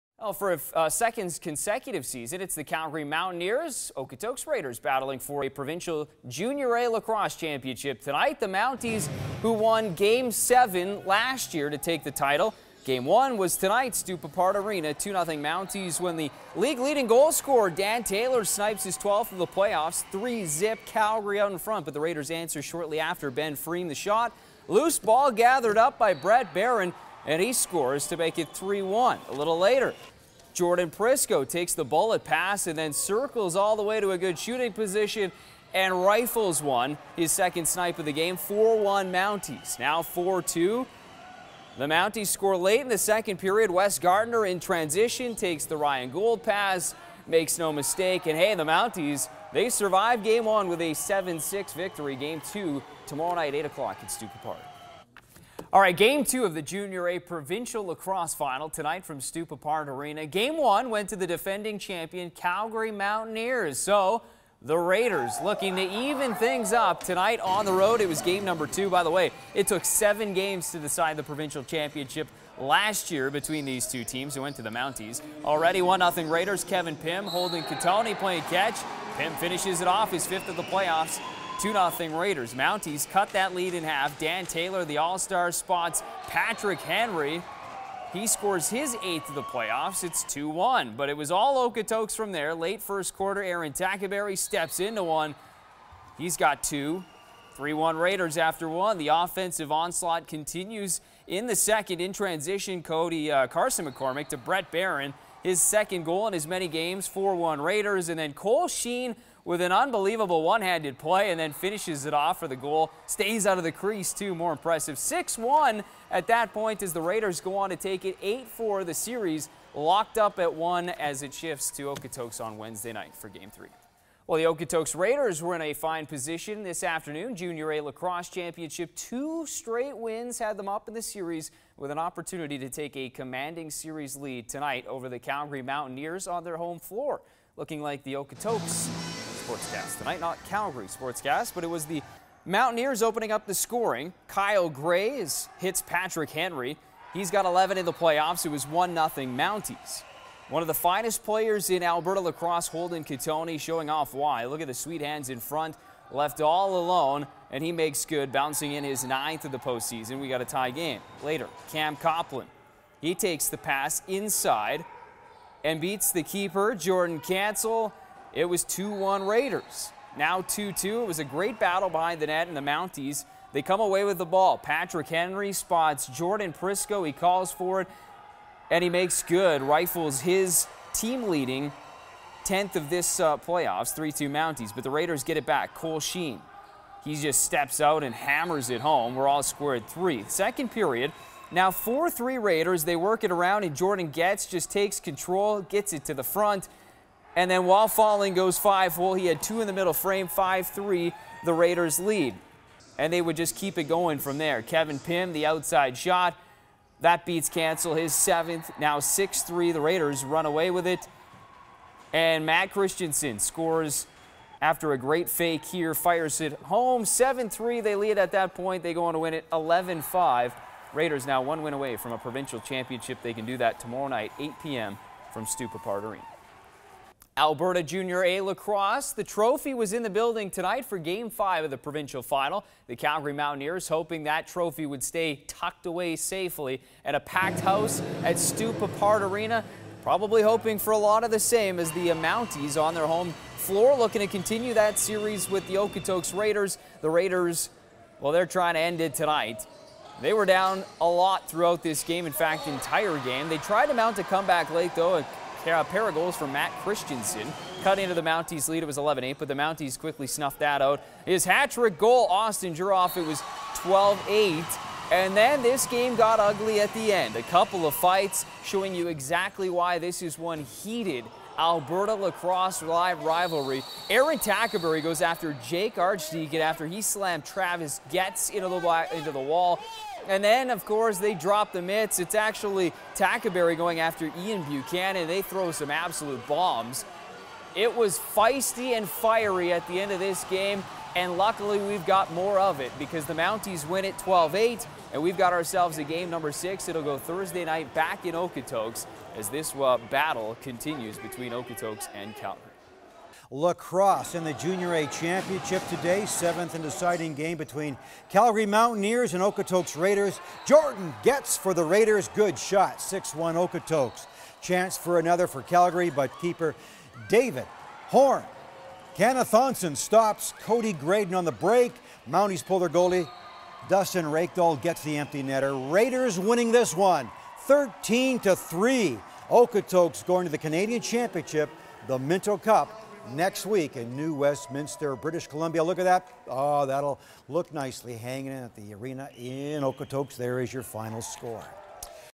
RMLL Final
For a second straight season, the provincial Junior A lacrosse championship was decided by two southern Alberta clubs. The Okotoks Jr. Raiders finally eliminated the Calgary Mountaineers after a seven game battle, advancing to the 2013 Minto Cup.